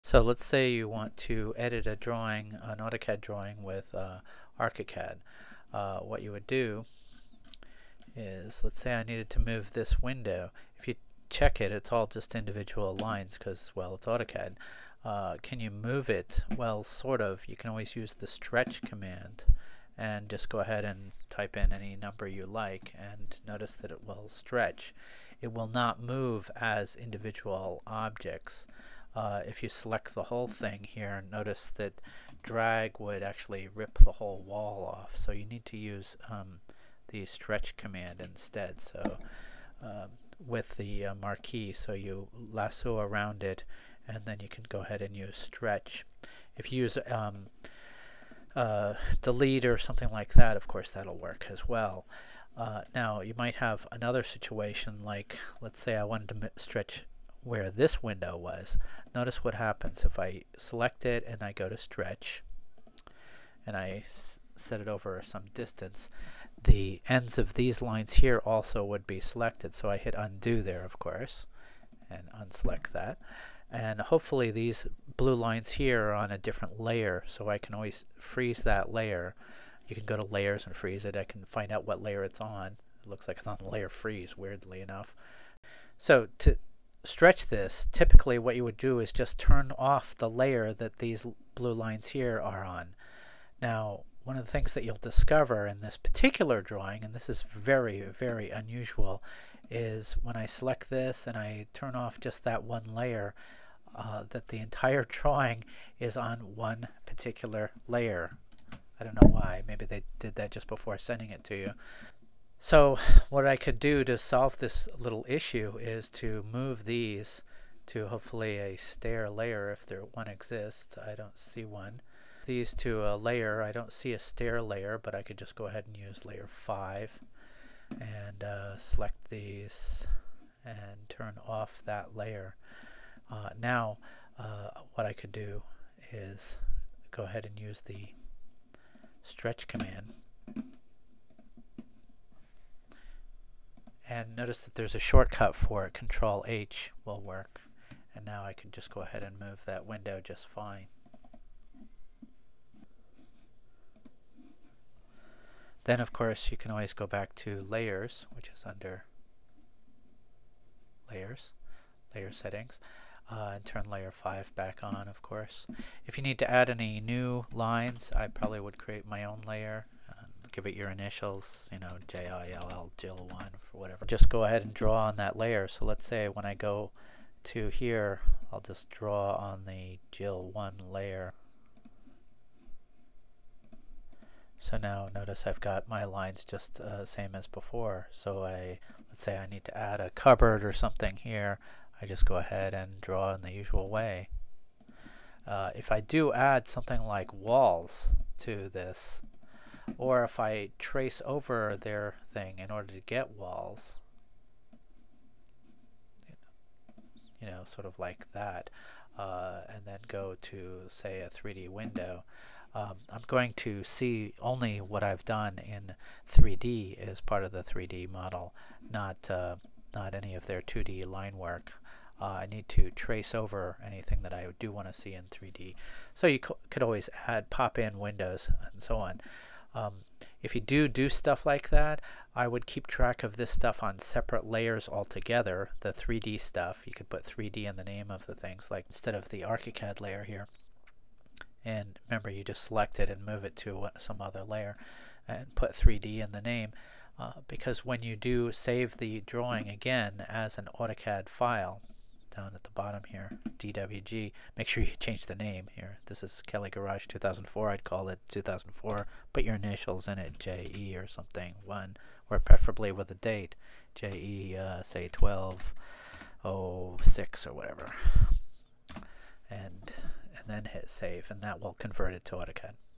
How to edit AutoCAD drawings with ArchiCAD
Just need to make some simple 2D changes to an AutoCAD drawing, without creating a whole BIM 3D model in ArchiCAD.  This simple video shows how.